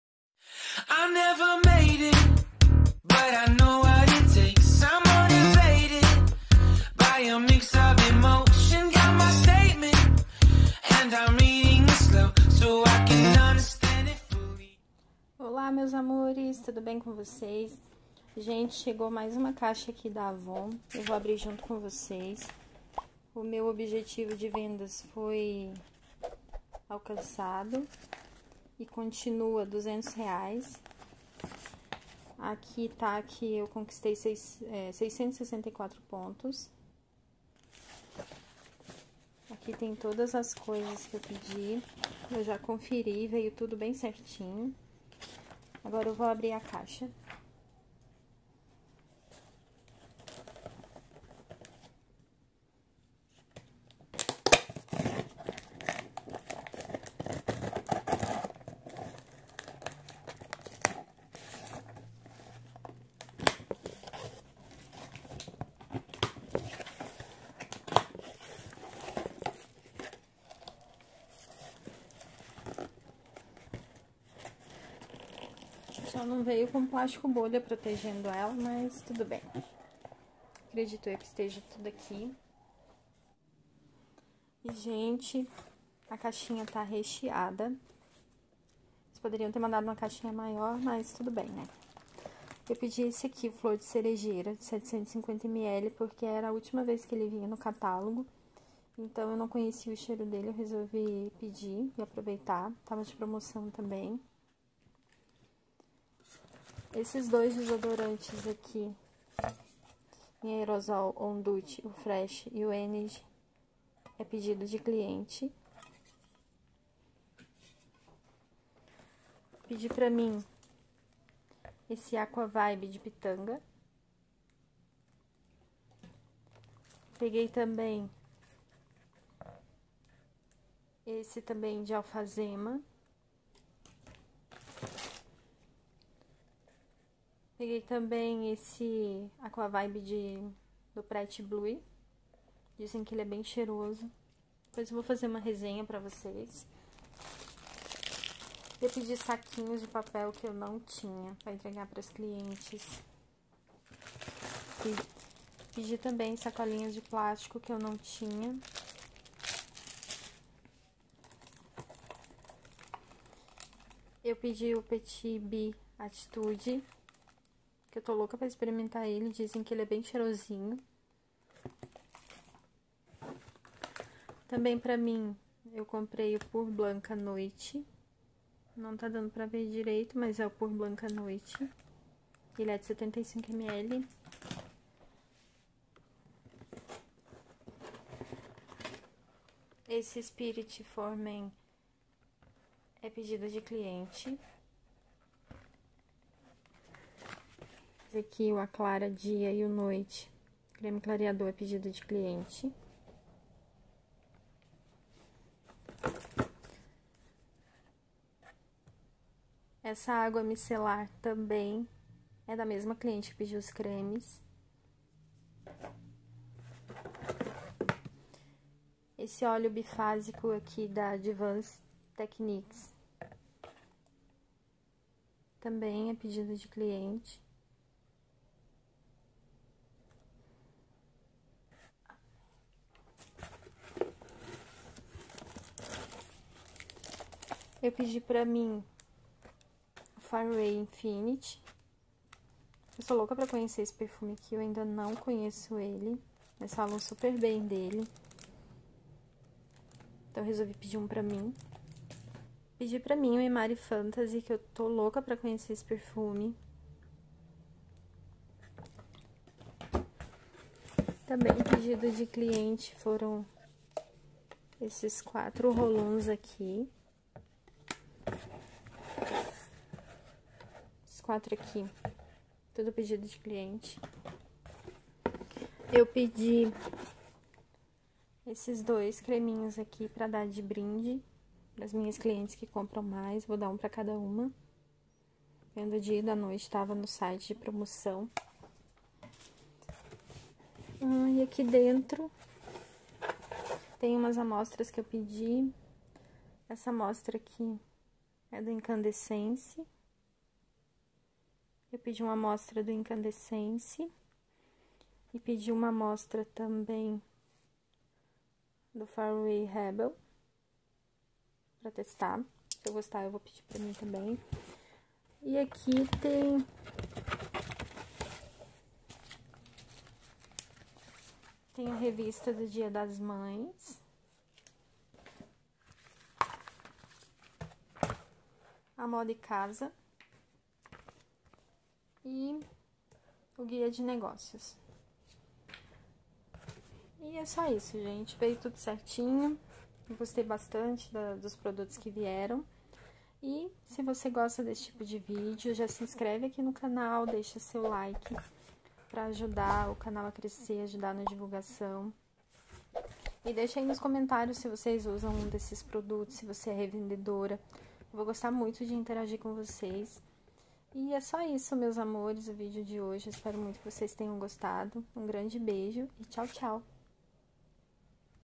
ABERTURA DE CAIXA DA AVON.CAMPANHA 06/20
Olá Amores!No vídeo de hoje eu trago a abertura de caixa da Avon da Campanha 06/20.Veio muita coisa bacana.Espero que gostem do vídeo,e se gostarem deixem seu Like e se inscrevam no canal.Bjs e até o próximo Vídeo. Me sigam no Instagram 🌷.💋👩🏻‍🦰🔥😘avon